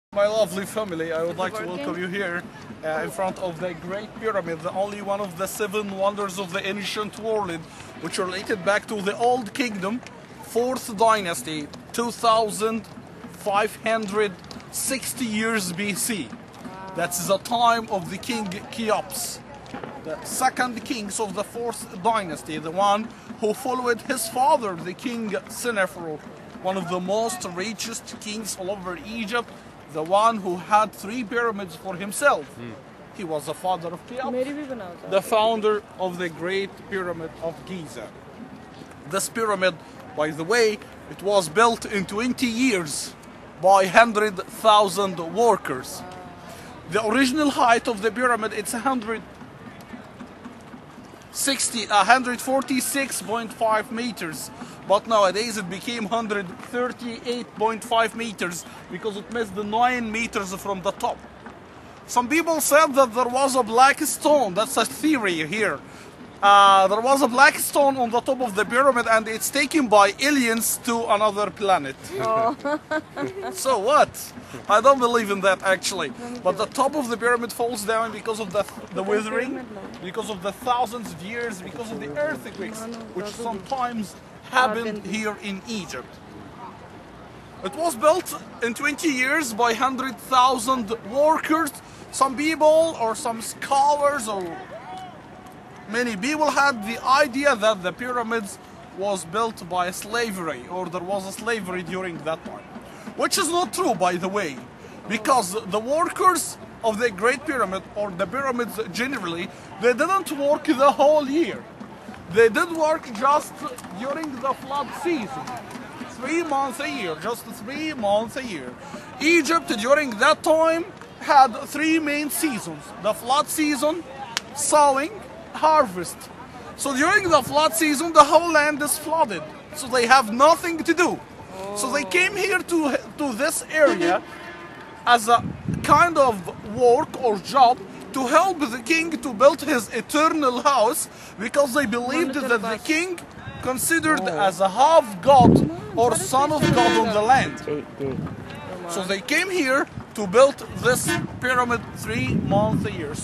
Pyramids + info pt.1
Our tour guide talking about the pyramids